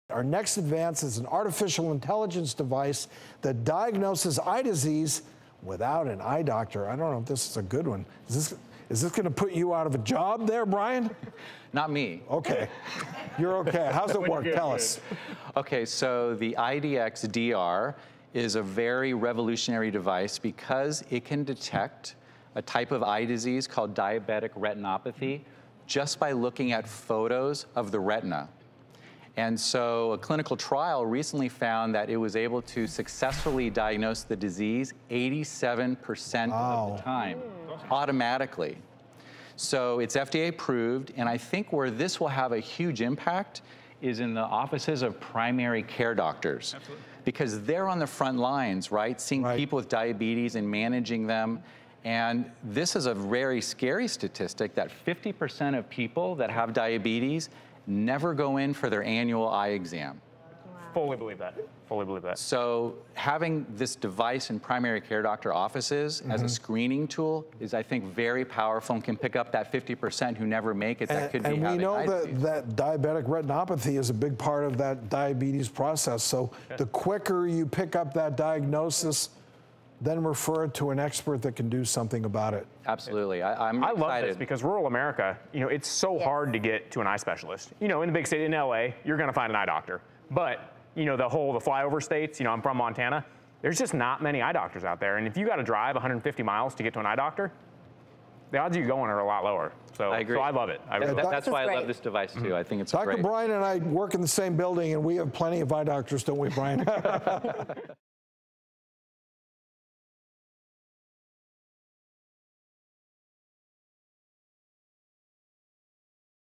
New Device Detects Eye Disease with Just a Photo?
Ophthalmologist Dr. Brian Boxer Wachler discusses a new device which may be able to detect an eye disease related to diabetes with simply a photo of the eyes.